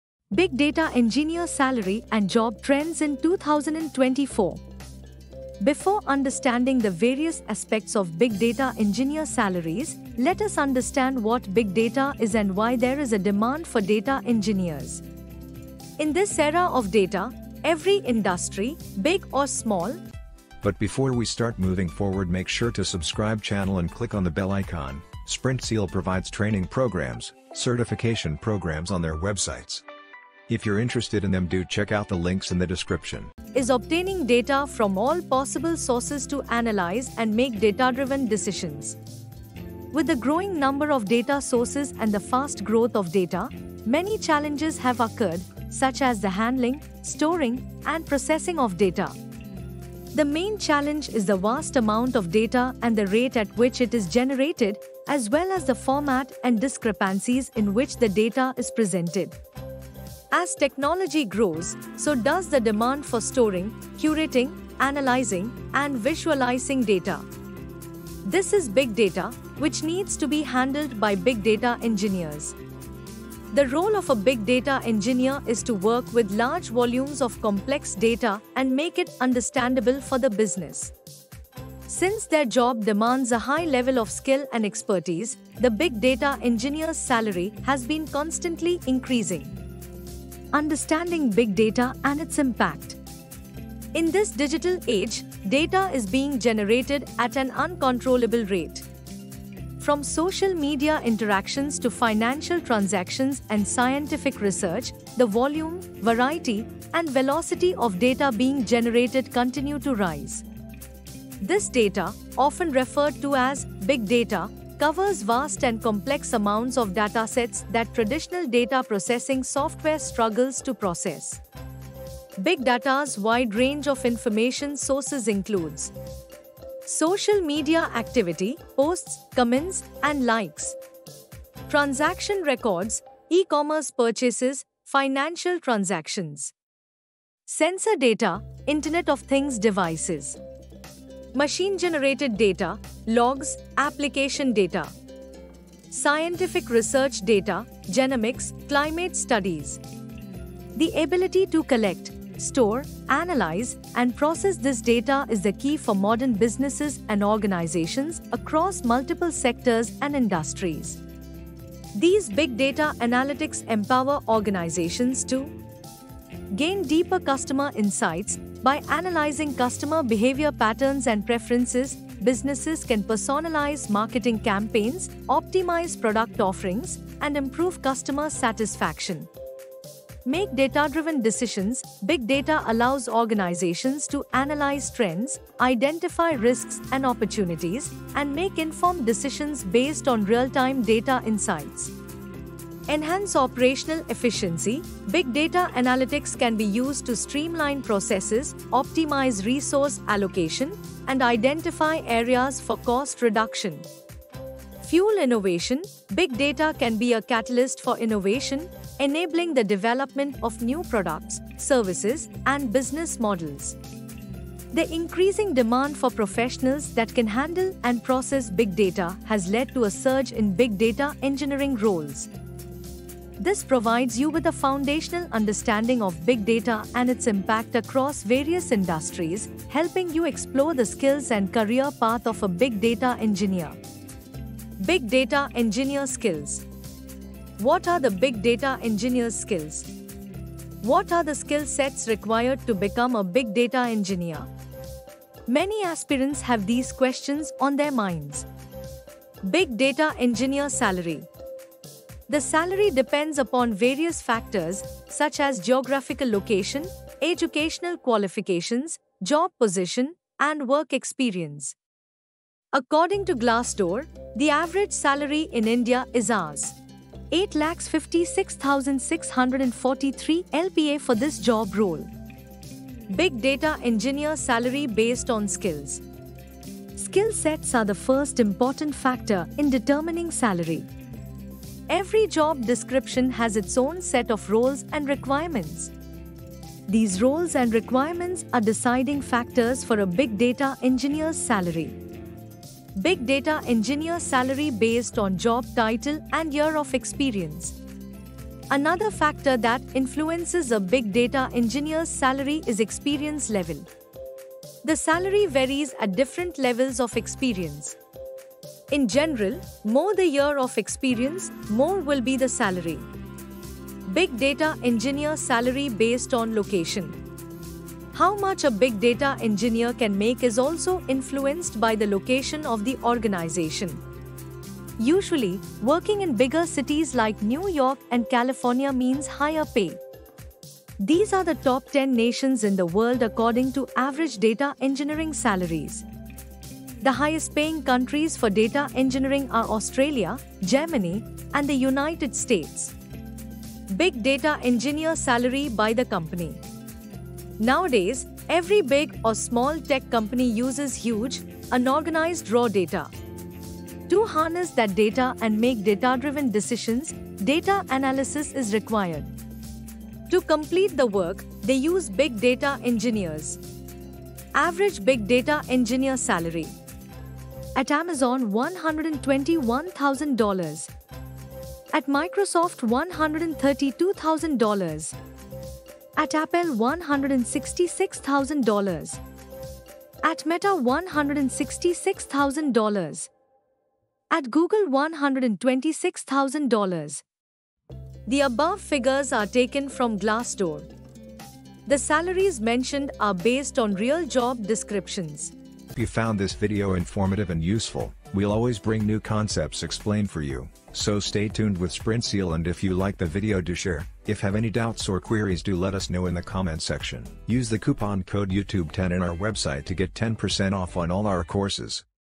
Big Data Engineer Salary and Job Trends in 2024
Here is Sprintzeal's video on What is Data Visualization A Comprehensive Guide
"Here is the link to read more about blog:"

Before understanding the various aspects of big data engineer salaries, let us understand what big data is and why there is a demand for data engineers.

In this era of data, every industry, big or small, is obtaining data from all possible sources to analyze and make data-driven decisions.

With the growing number of data sources and the fast growth of data, many challenges have occurred, such as the handling, storing, and processing of data.

The main challenge is the vast amount of data and the rate at which it is generated, as well as the format and discrepancies in which the data is presented.

As technology grows, so does the demand for storing, curating, analyzing, and visualizing data. This is big data, which needs to be handled by big data engineers.

The role of a big data engineer is to work with large volumes of complex data and make it understandable for the business. Since their job demands a high level of skill and expertise, the big data engineer's salary has been constantly increasing.

Let’s start with some basic questions

what you will learn here :
A. Introduction
1. data engineers.
2. big data engineer's salary
B. Understanding Big Data and Its Impact
1. Gain deeper customer insights
2. Make data-driven decisions
3. Enhance operational efficiency
4. Fuel innovation
C. Big Data Engineer Skills
D. Big Data Engineer Salary
1. Big Data Engineer Salary Based on Skills
2. Big data engineer salary based on job title and year of experience
3. Big data engineer salary based on location
4. Big data engineer salary by the company